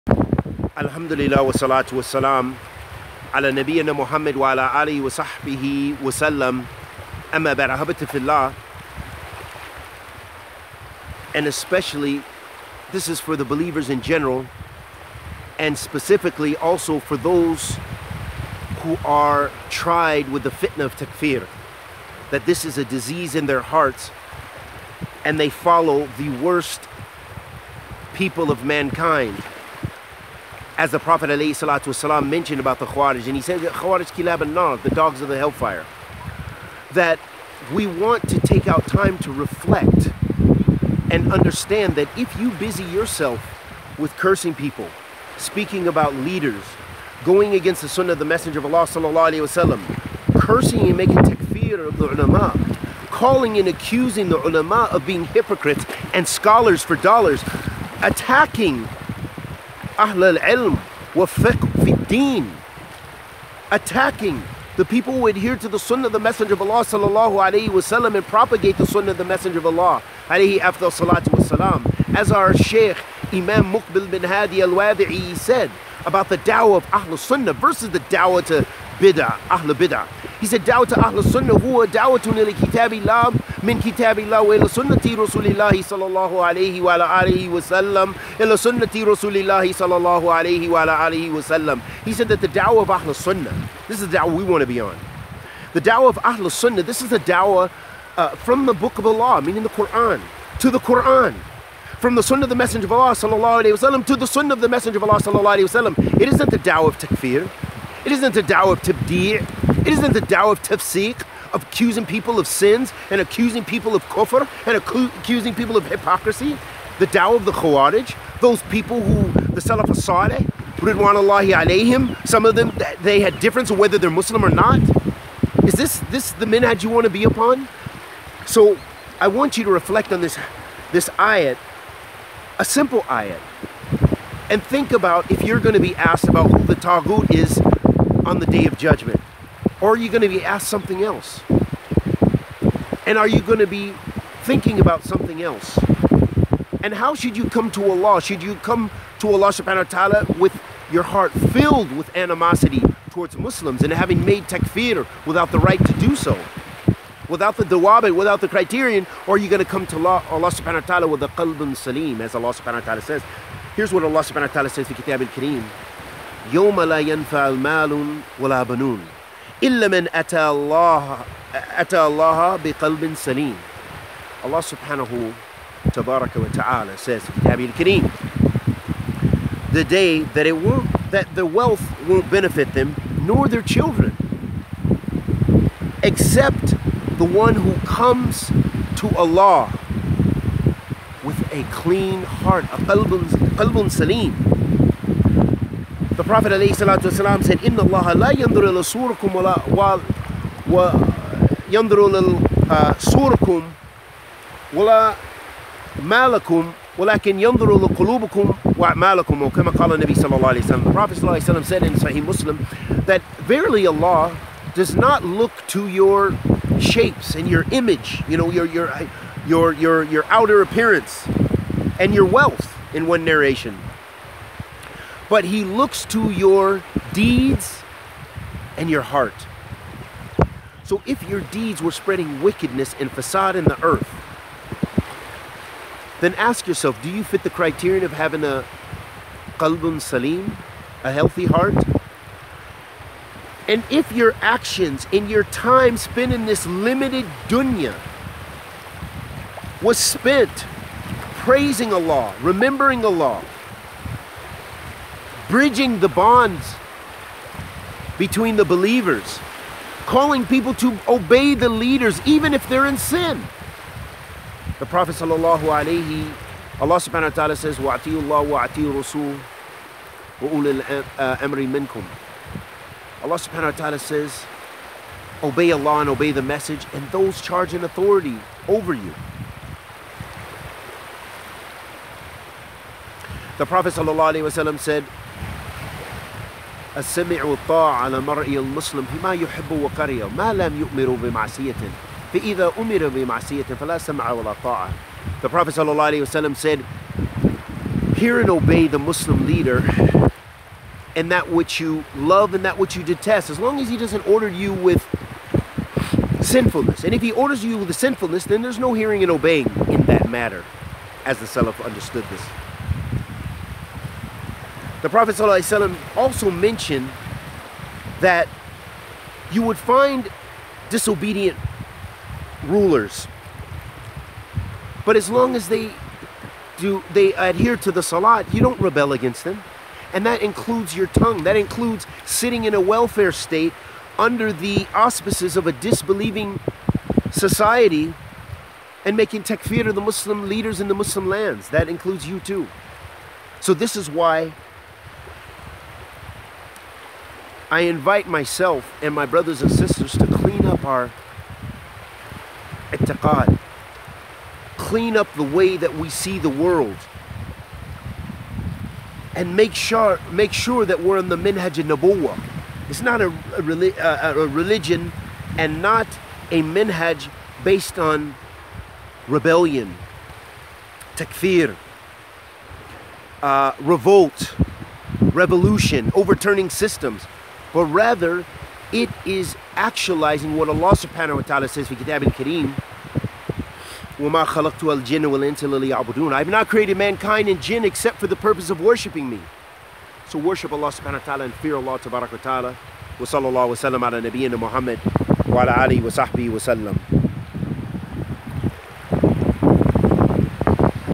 Followers of Abdullah El-Faisal Check Your Hearts!
Faith is in the heart, actions of the limbs and statements of the tongue and we will not be asked about our leaders and who is taught or not. Focus on your priorities and rectify yourself before attempting to politicise Islam.